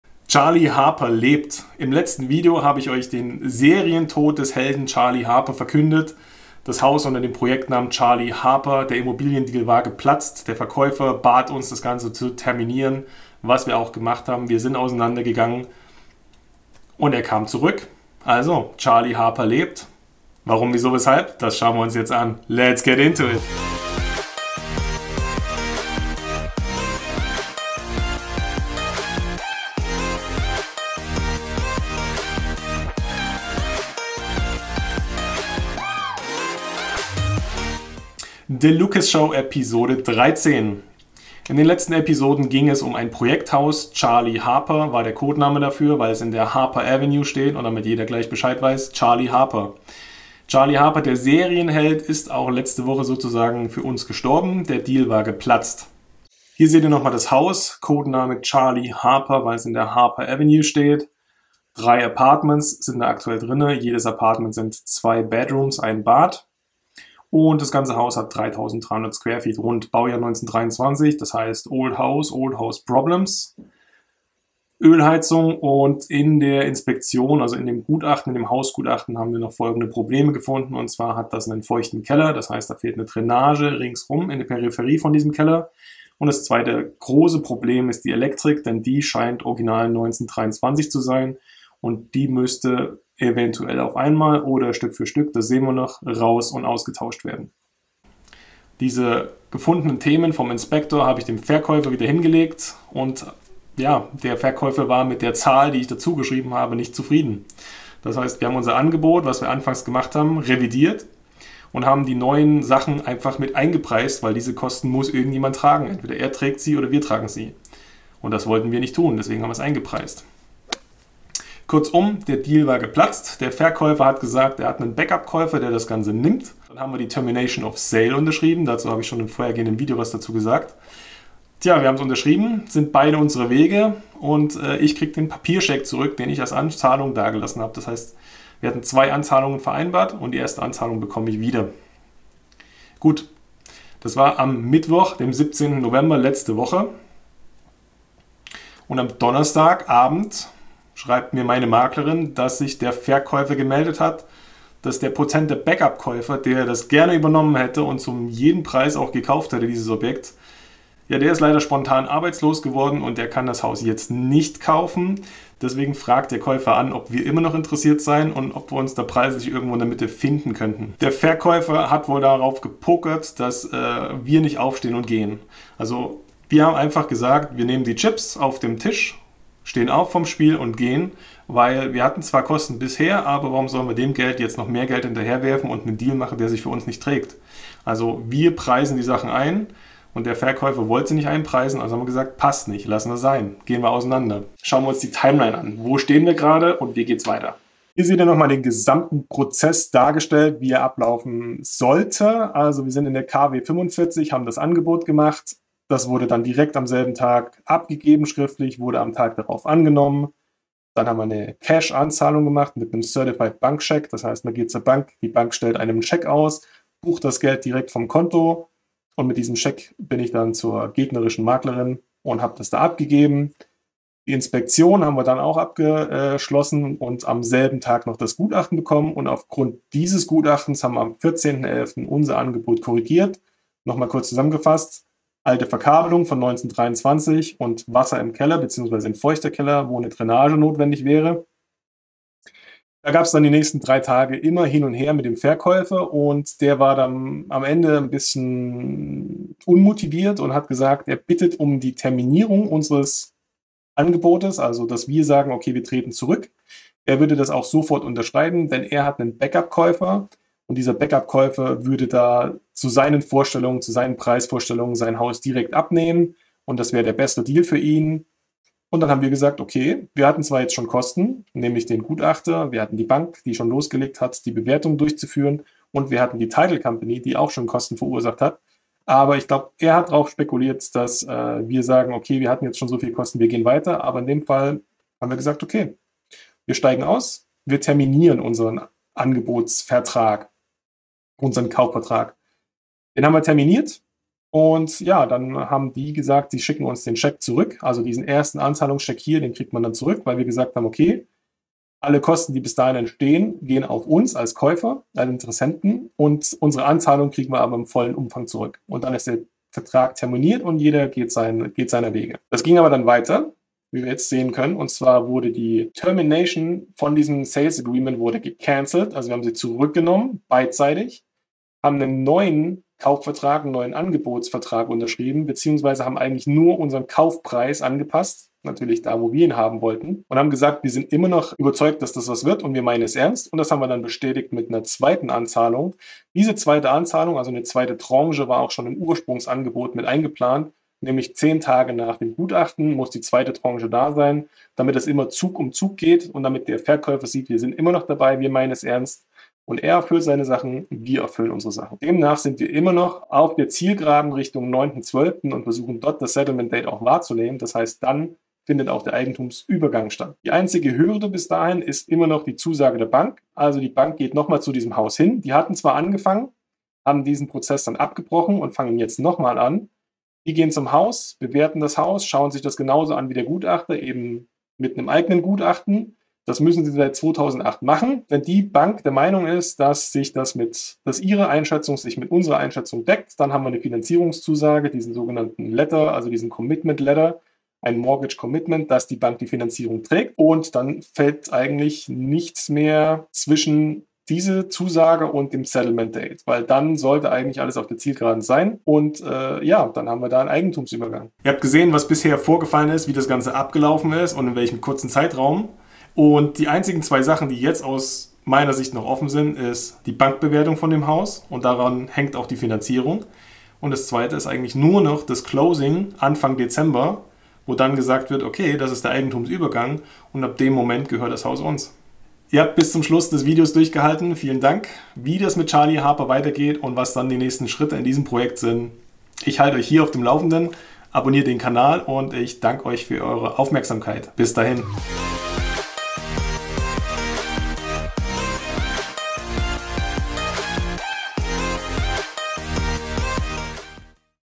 Deal für Mehrfamilienhaus in Amerika erfolgreich verhandelt! (Ep. 13)
Der Immobilien Deal für das Mehrfamilienhaus mit dem Codenamen “Charlie Harper” ist doch nicht mehr geplatzt. Der Verkäufer kommt zurück. Schau selbst...
⬇️  Klicke "Mehr Ansehen" für mehr Videoinformationen
Glücklicherweise hat es sich der Verkäufer des Hauses anders überlegt. Er bot uns das Haus erneut an. Im Gegensatz zur Verhandlungsrunde 1 wurden die Themen aus dem Hausgutachten / Inspektion erfolgreich nachverhandelt und wie selbstverständlich mit eingepreist. Endlich: Der Kaufprozess geht weiter. Sieh selbst...